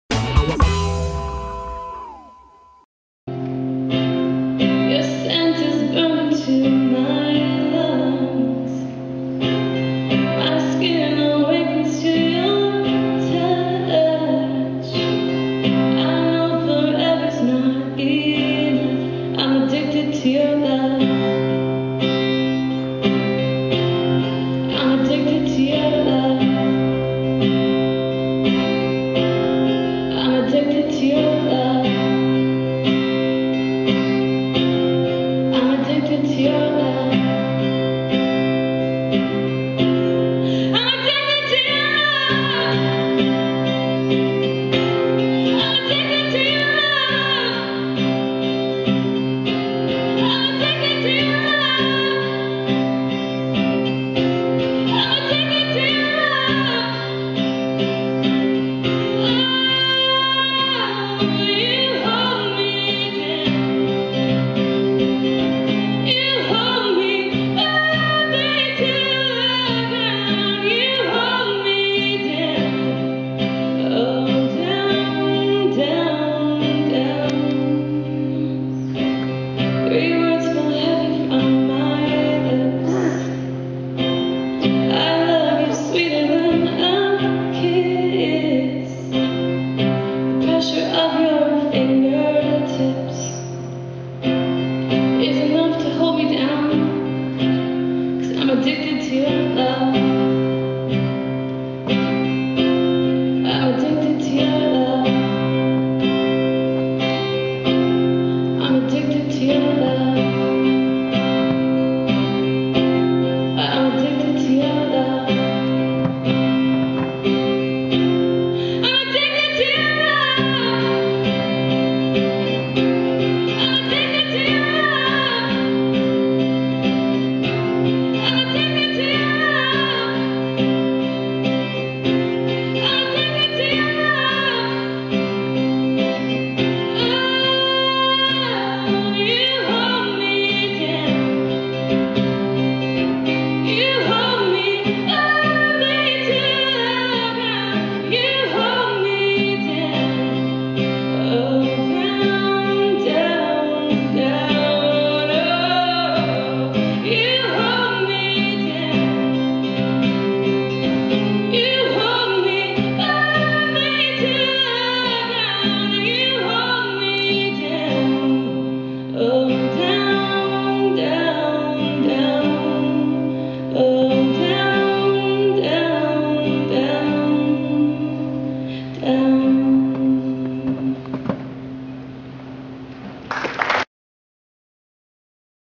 Land the Big Gig: Waiting to Run
Waiting To Run is just Sue Smith of Milwaukee with her acoustic guitar, and her honest, melodic, catchy, heartfelt and engaging songs.